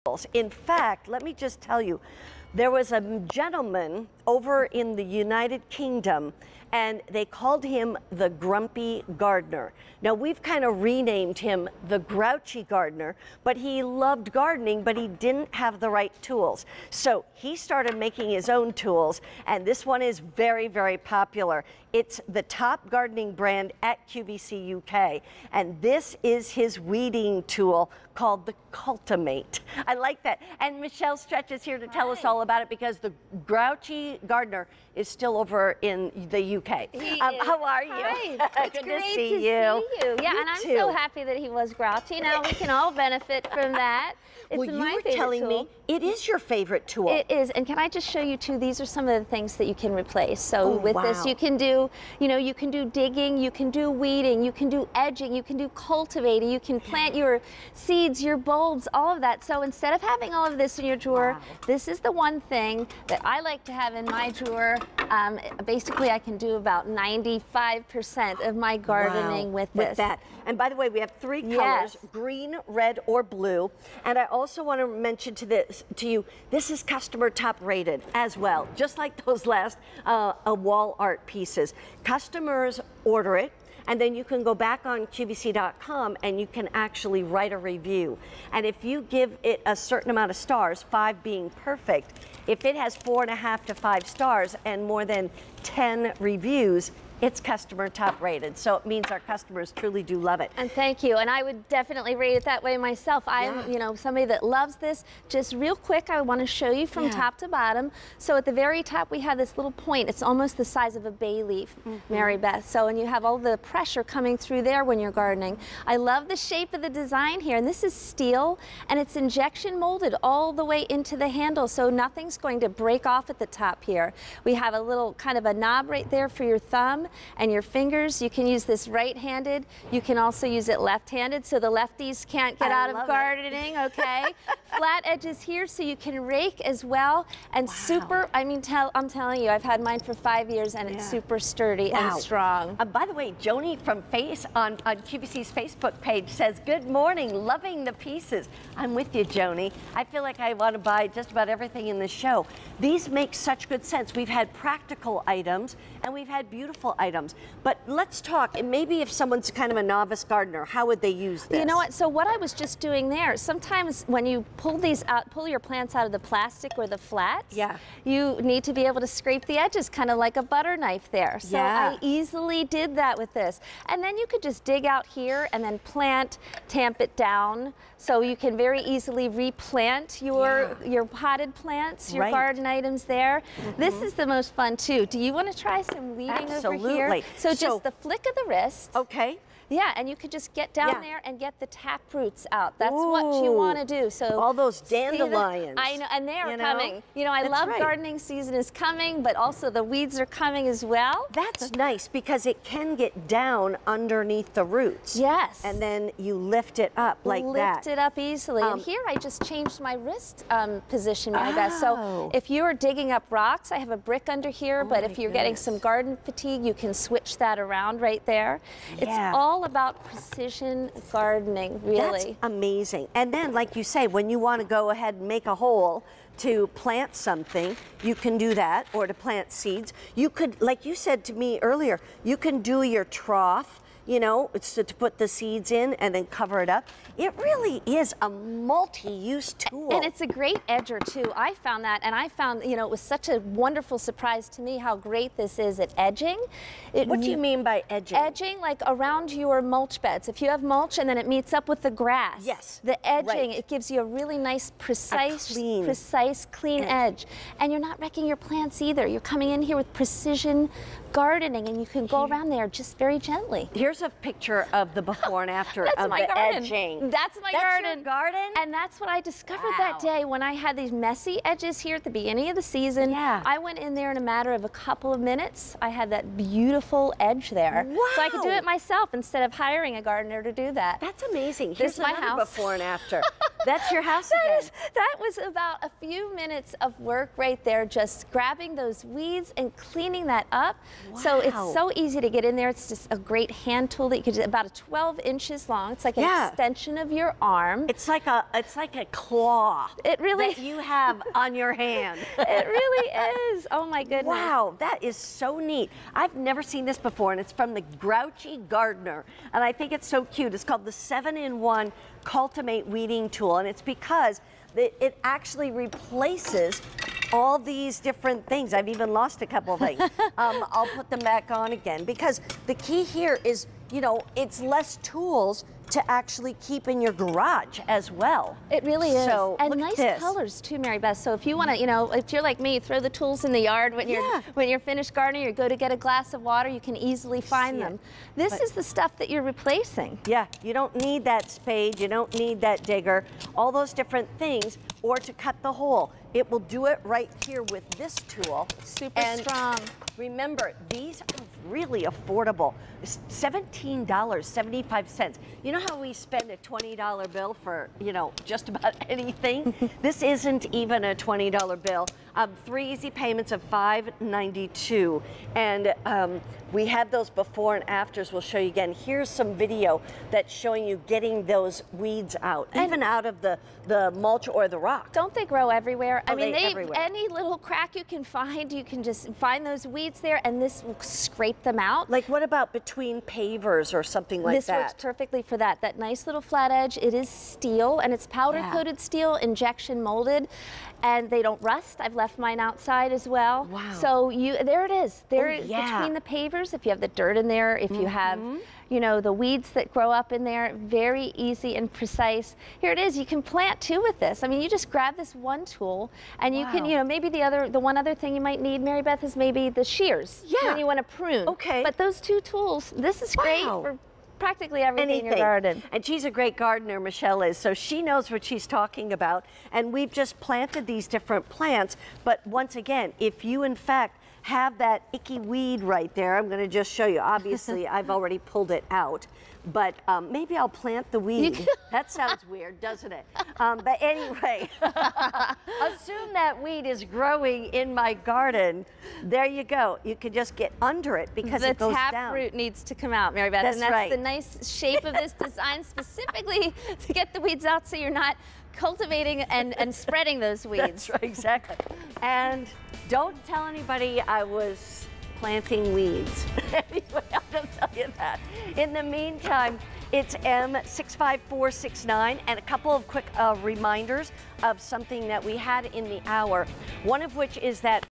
7-in-1 Cultimate Weeding Tool by Grouchy Gardener on QVC
7-in-1 Cultimate Weeding Tool by Grouchy Gardener

Weed, break up soil, create furrows, and plant with this clever gardening tool! From Grouchy Gardener.